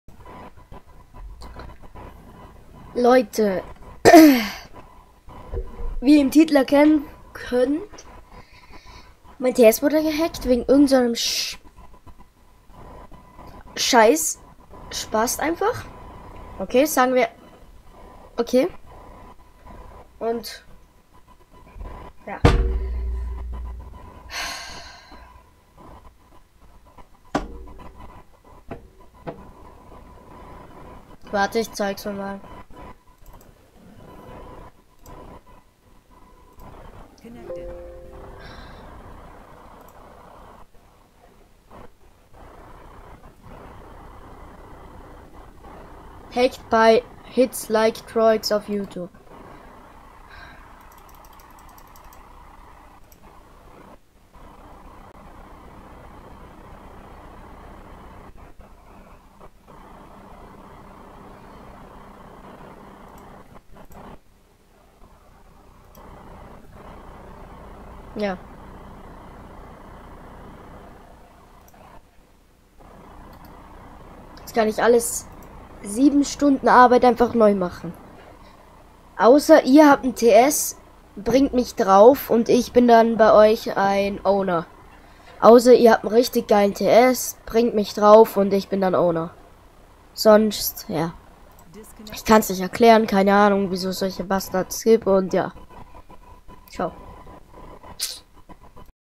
Mein TS wurde gehackt!!!!!!!:(!!!!!!:C
Bitte wen ihr einen TS habtz dann bitte gebt mir owner und schreibt die ip danke